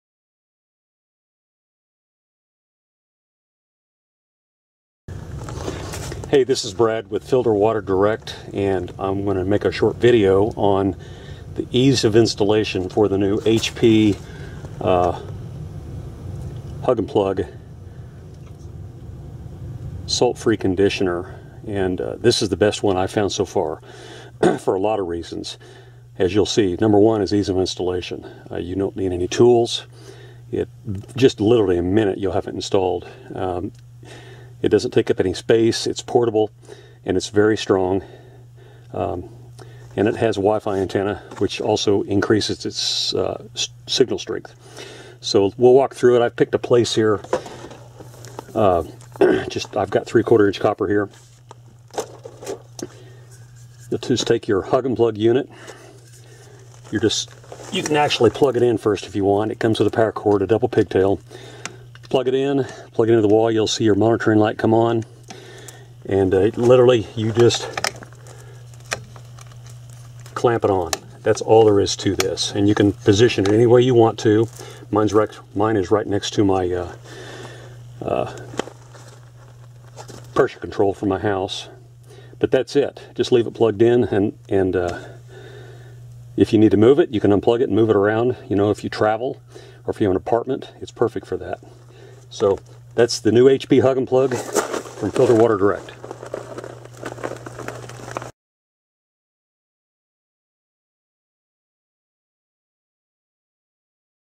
HD Hug-N-Plug Solid State Digital Hard Water Water Conditioner Installation
The HD Hug-N-Plug Digital electronic hard water conditioner requires no tools and minimal space, works with plastic, pex, and copper piping, and installs in about one minute. It's very portable and can be moved to apartments, RVs, and many applications.